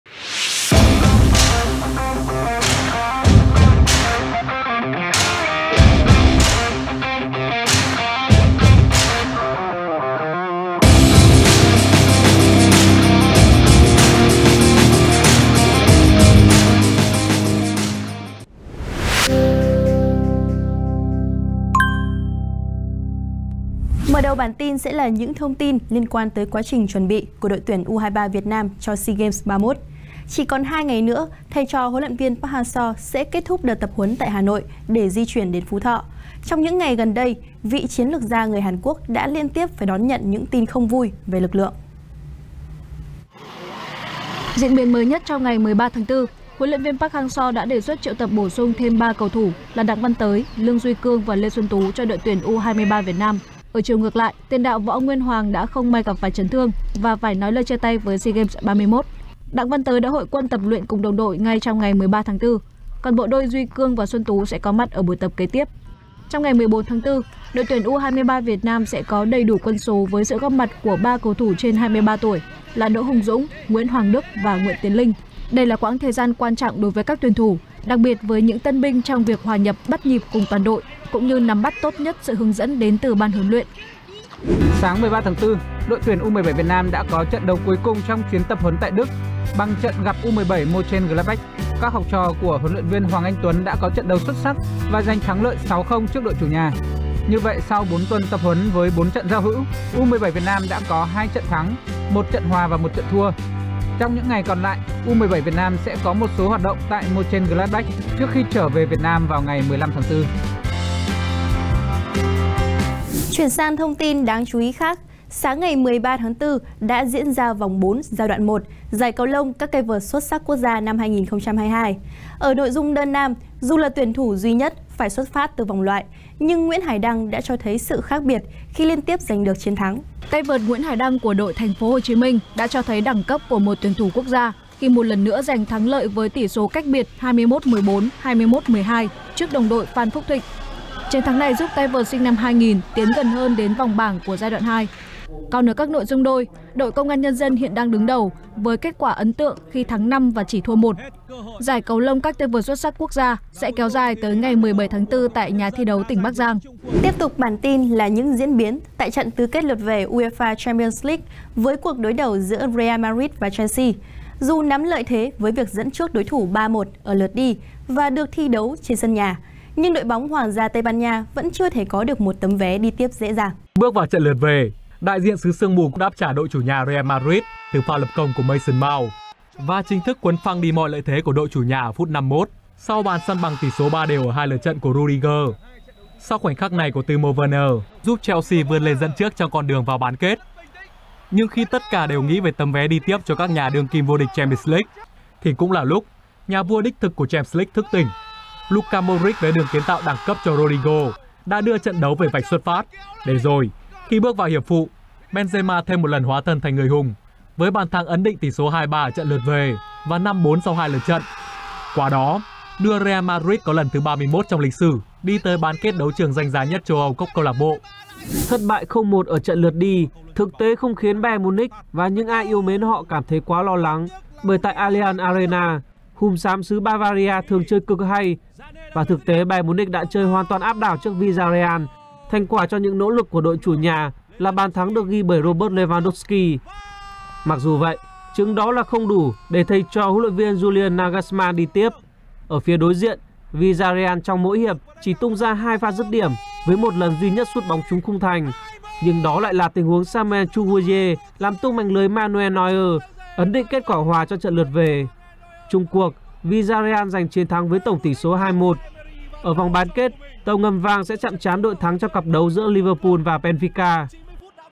THỂ THAO 14/4: REAL MADRID VS MANCITY TẠI BÁN KẾT, BENZEMA LIỆU CÓ TỎA SÁNG?
Một số tin chính được để cập:
- ĐT U23 Việt Nam bổ sung 3 cầu thủ
- Thai League thay đổi lịch thi đấu tạo điều kiện cho ĐT U23
- Malaysia chỉ dùng cầu thủ dưới 23 tuổi cho Sea Games
- U17 Việt Nam kết thúc chuyến tập huấn tại Đức với chiến thắng trước U17 Monchengladbach
- Cầu lông: Nguyễn Hải Đăng tiếp túc giành chiến thắng tại vòng 4 giải các tay vợt xuất sắc 2022
- Chung kết vòng loại giải Junior Davis Cup: Các tay vợt Việt Nam nhận thất bại đầu tiên
- Lượt về tứ kết UEFA Champions League: Real Madrid tiếp tục vượt khó nhờ Benzema